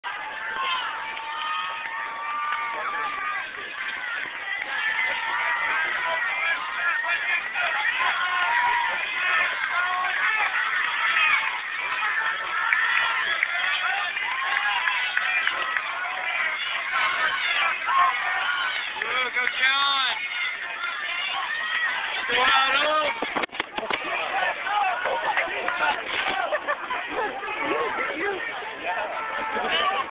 Camera Guy Gets Ran Into at Cross Country Meet
A runner runs into the camera man at a Cross Country race